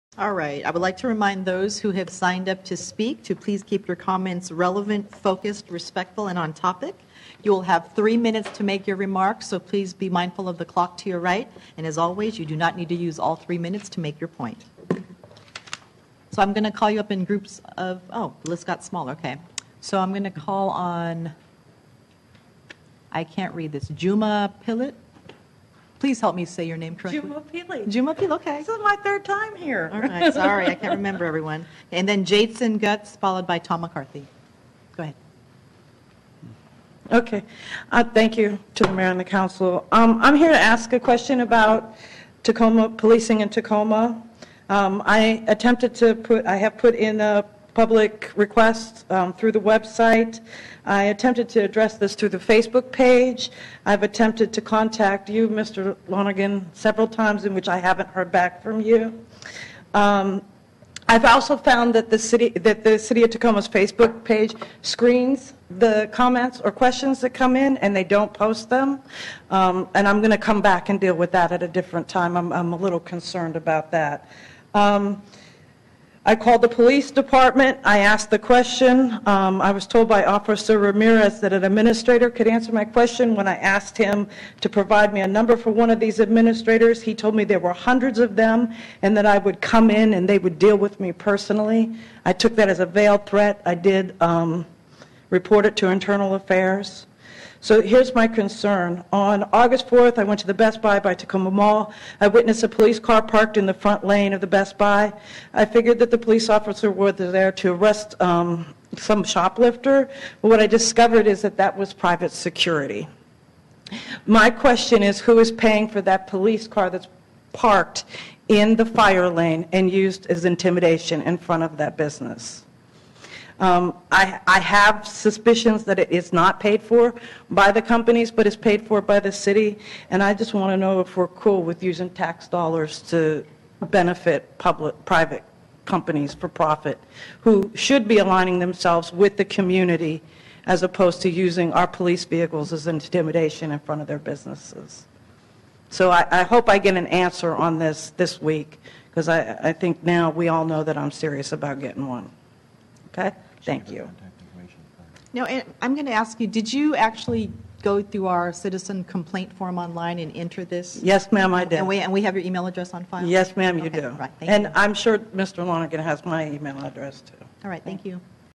Tacoma City Council Public Forum - Policing In Tacoma?
On Aug 4th I went to the Best Buy by the Tacoma Mall. I witnessed a police officer and her car parked in the fire lane. I assumed that she might have been there to arrest a shoplifter, So I waited... What I discovered is that she sat at the front door for about an hour on her cell phone and yucking it up with staff. At this point I called for a supervisor to tell me what was going on. The manager told me she was private security because of a shooting 4 years ago. I asked both the manager and the officer who was paying for the police car parked in the fire lane? They didn't seem to know. I want to know how the city recoups the cost of using our tax payer supported police cars for private security jobs? I personally have a problem with private companies using the police vehicles parked in fire lanes as intimidation in the communities they are taking money from.